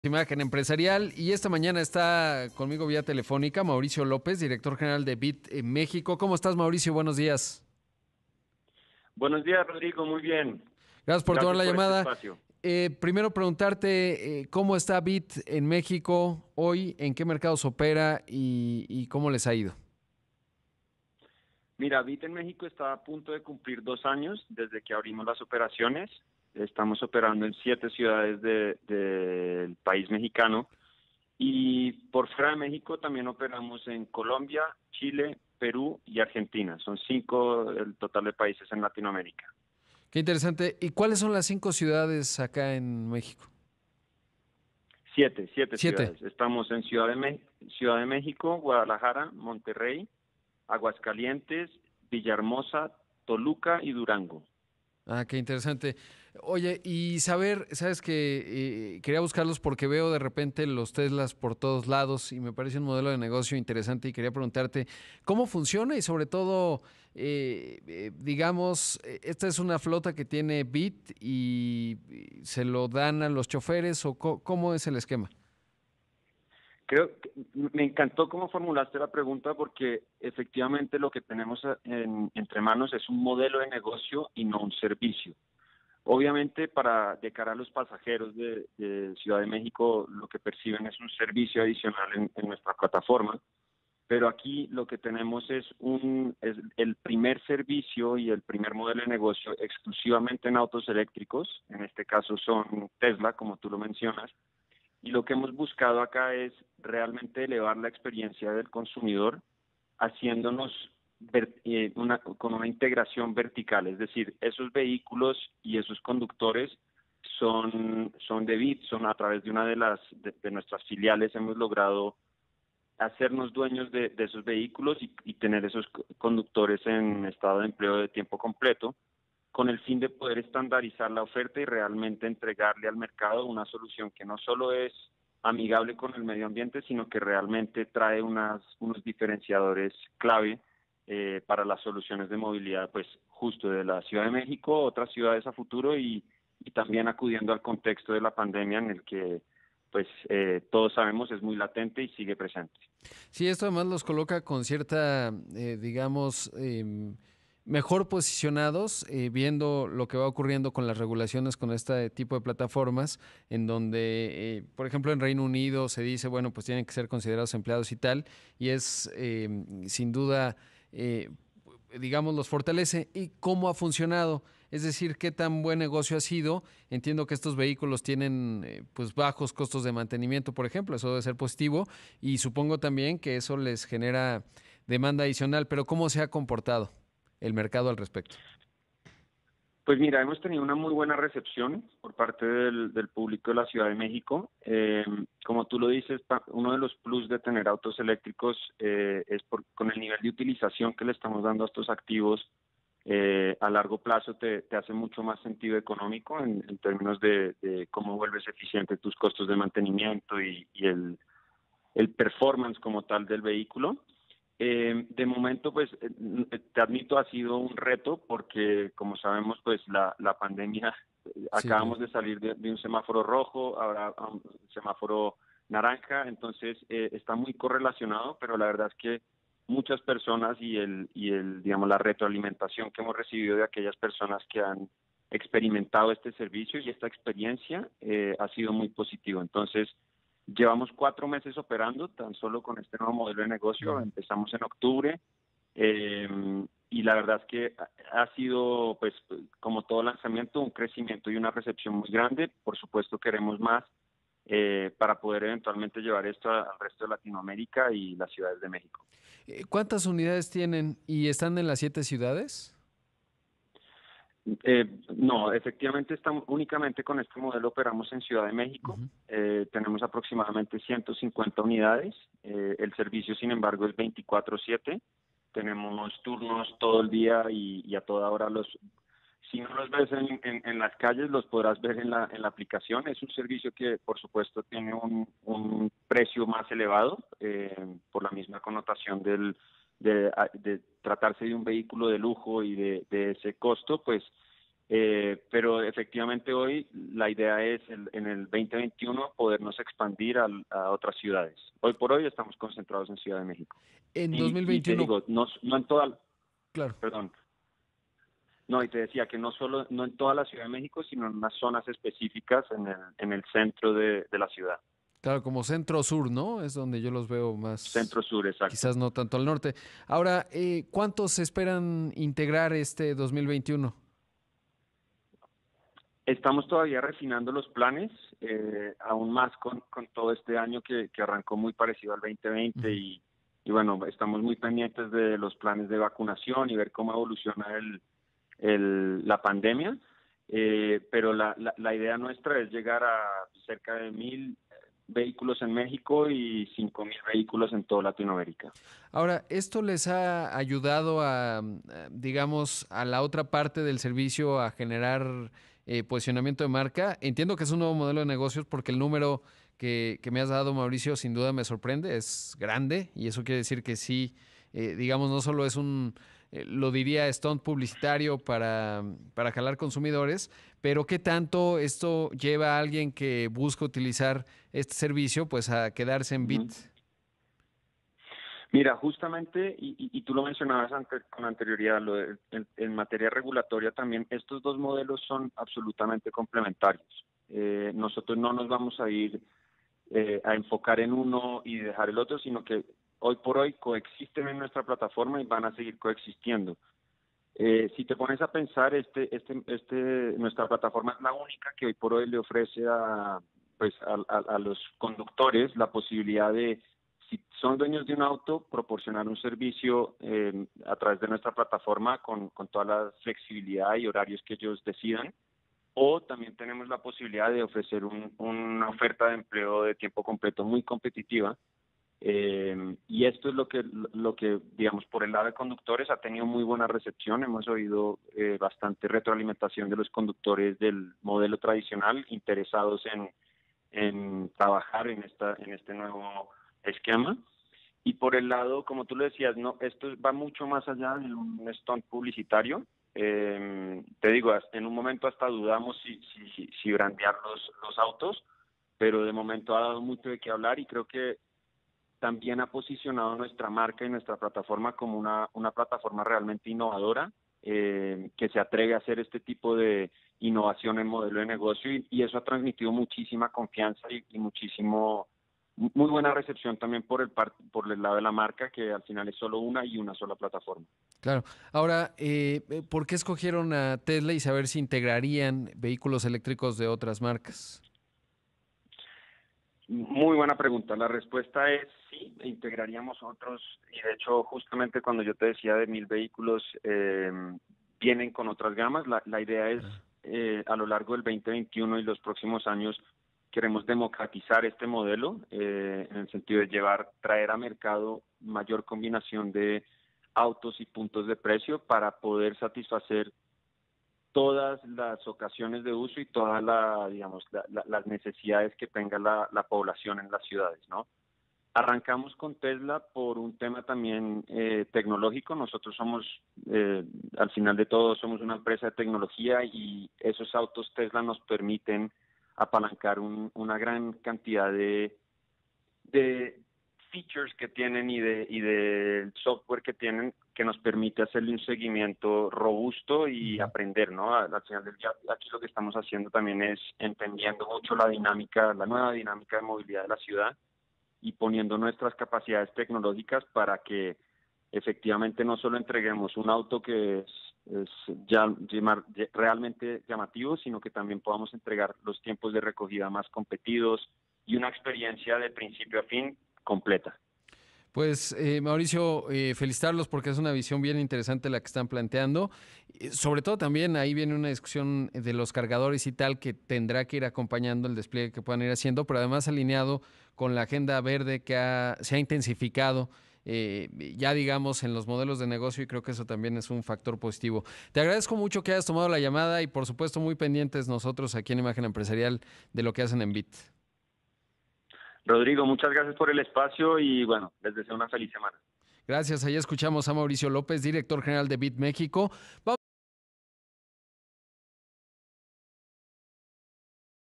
Beat México agrega flotilla de autos eléctricos en zonas clave de la CDMX
Mauricio López, director general de Beat México, se refirió en entrevista a sobre la flotilla de autos eléctricos que suman a sus servicios para zonas clave en la CDMX y que esperan replicar en otras ciudades del país.